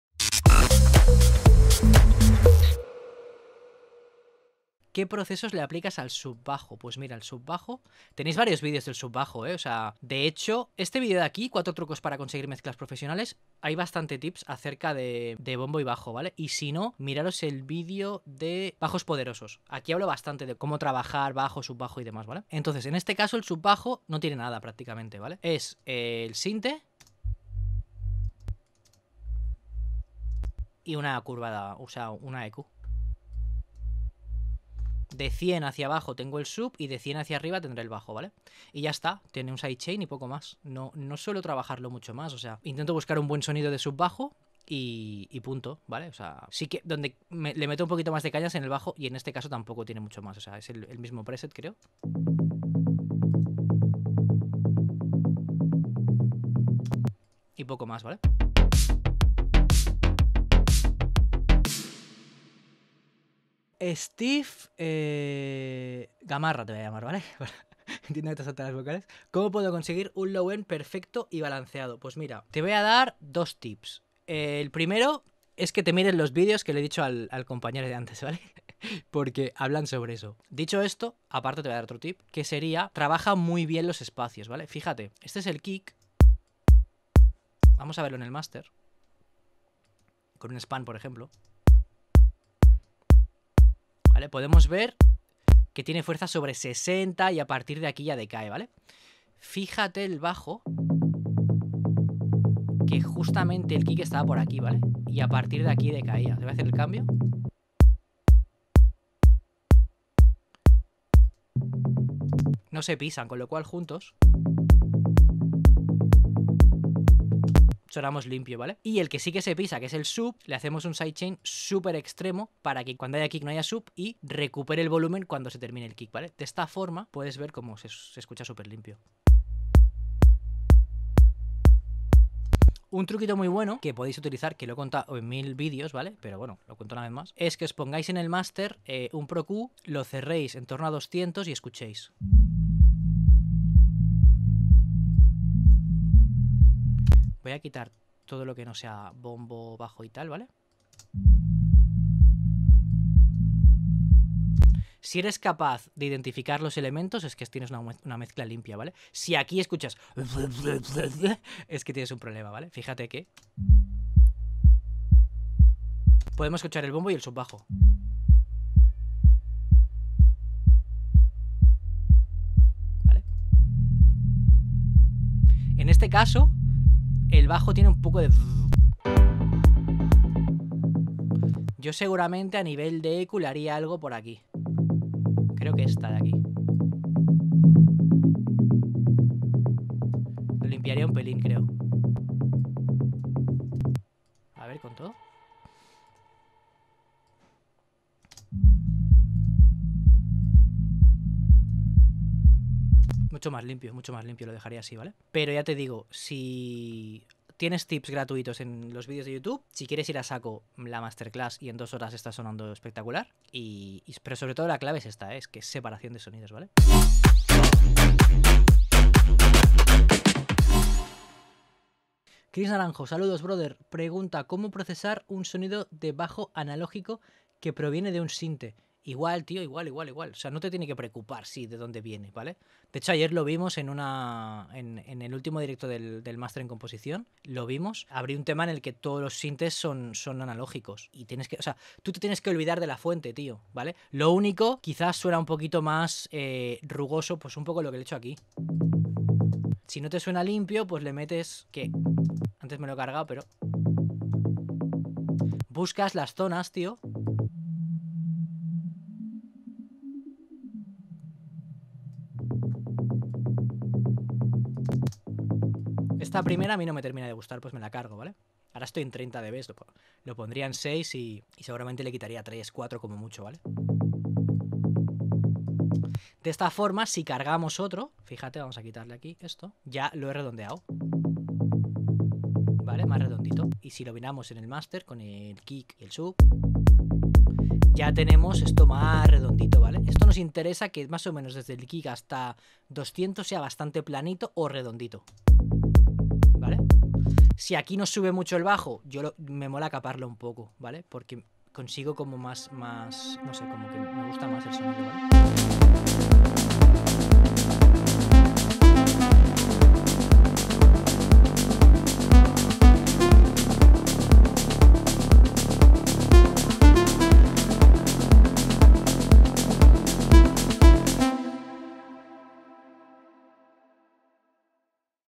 3 Trucos para MEZCLAR BOMBO y BAJO | Balancear KICK y BASS PROFESIONAL (Low End Limpio y Potente)
Descubre tres trucos para conseguir que la mezcla de tu bombo y tu bajo suene limpia, potente y definida. Como verás son tres pasos muy sencillos pero que si los aplicas bien te dan grandes resultados